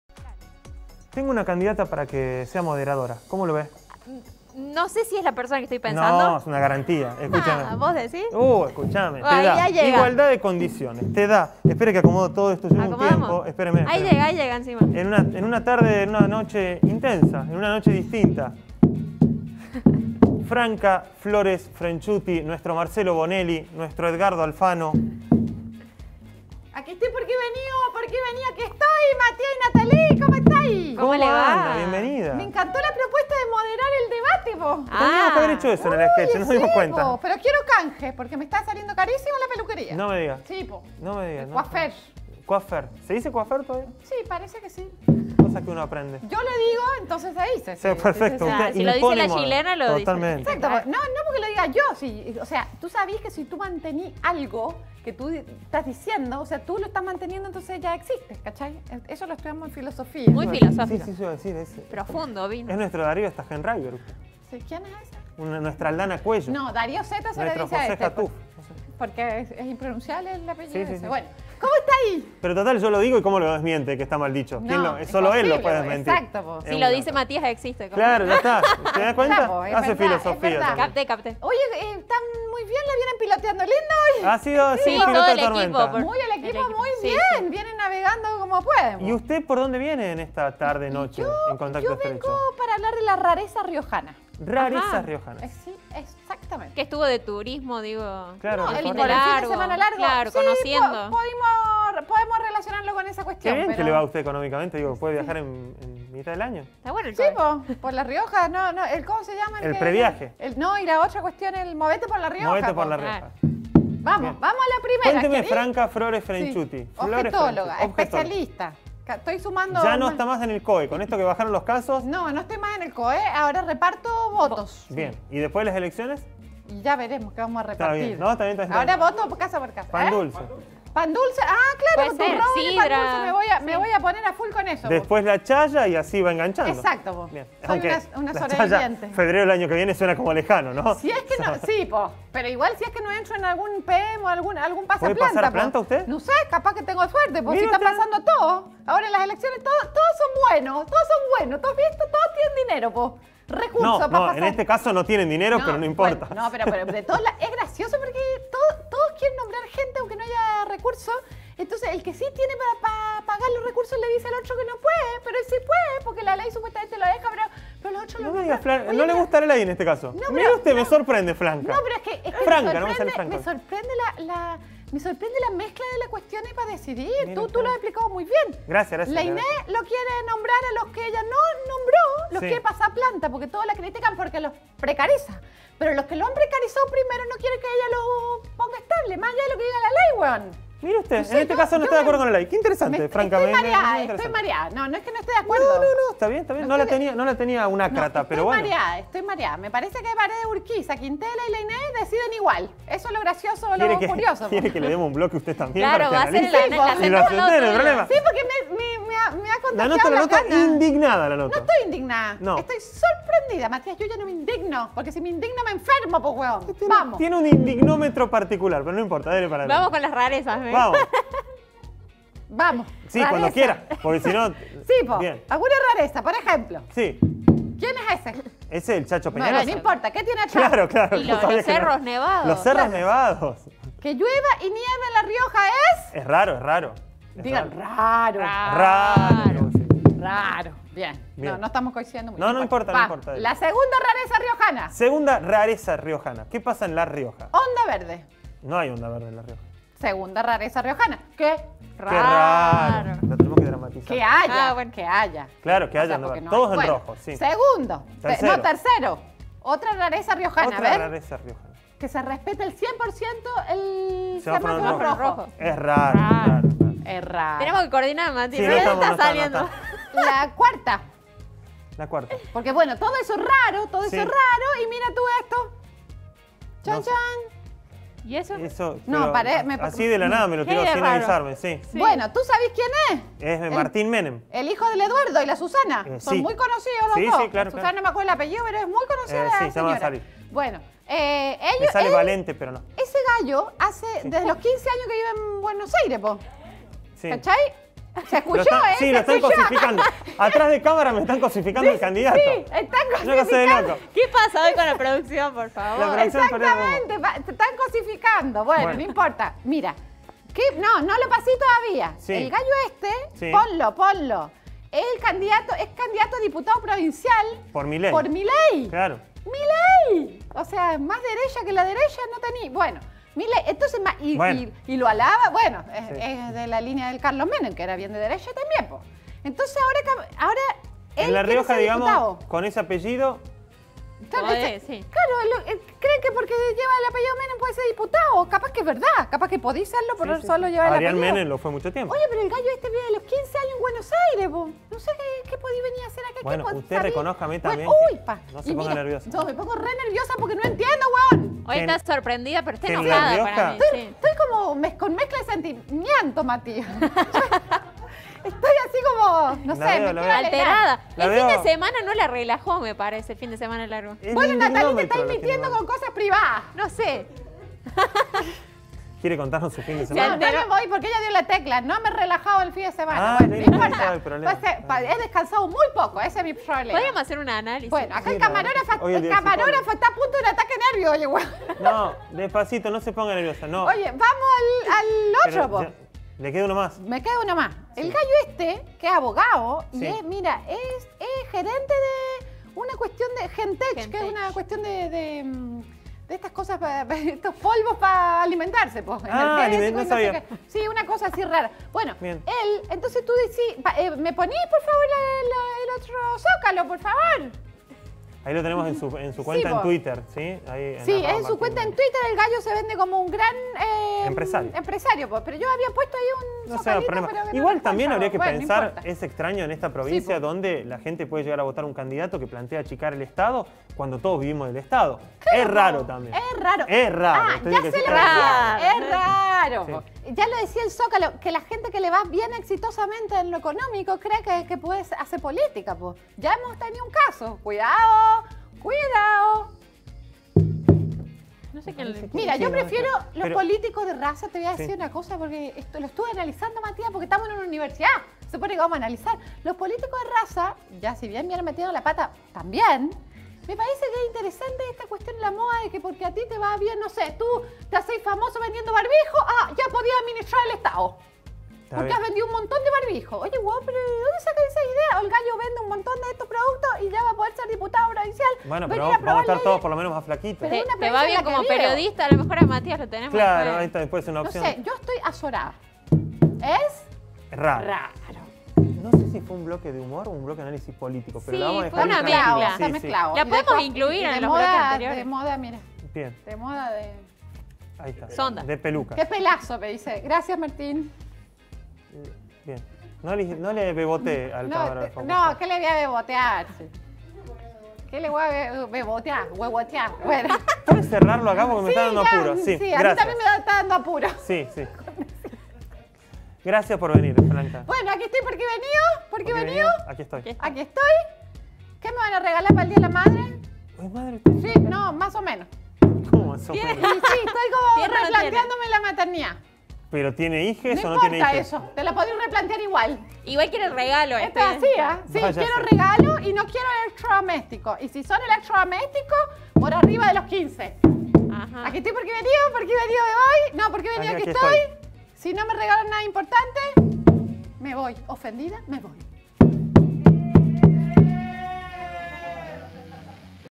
La Shilena: datos insólitos de los últimos días
Que encuentres onda verde, que los vehículos respeten el semáforo en rojo, que seas candidato a diputado por La Rioja y vivas en Buenos Aires desde los 15 años... estos y otros datos insólitos, nos cuenta Franca Flores Frenchutti.